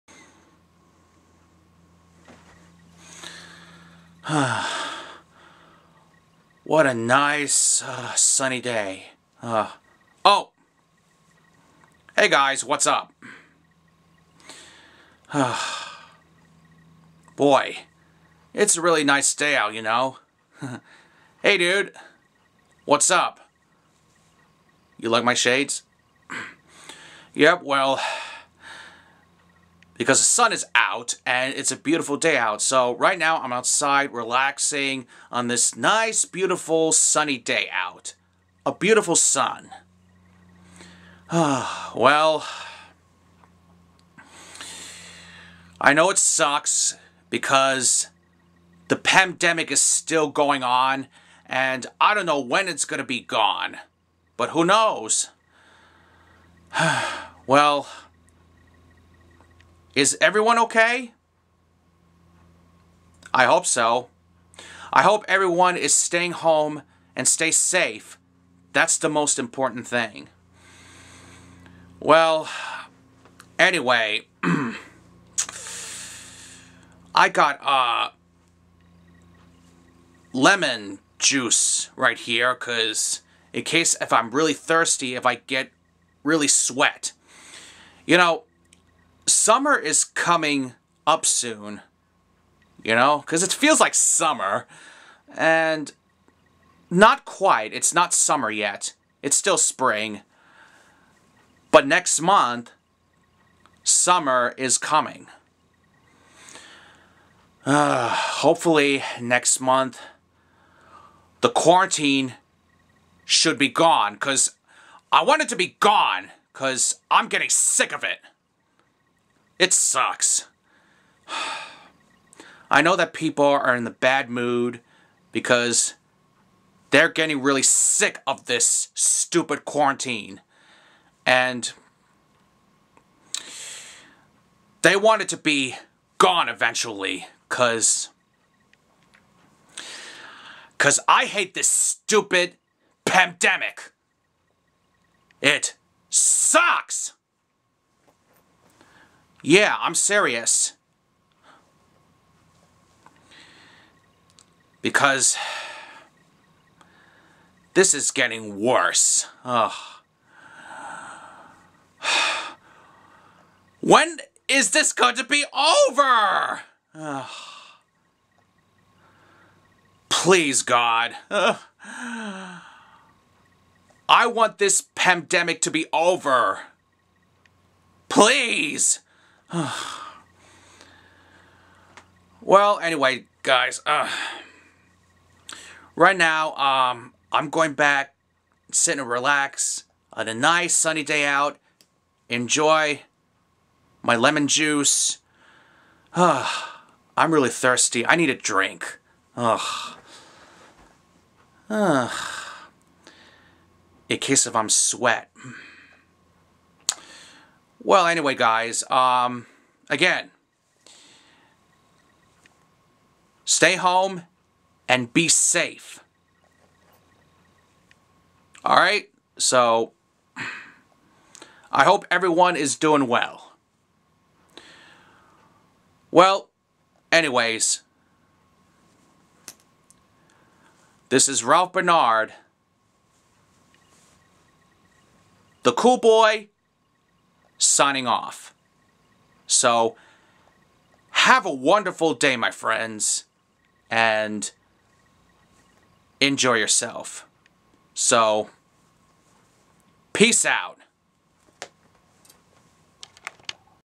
Cool Boy Is Relaxing
Ralph Bernard The Cool Boy is relaxing outside on a nice sunny day!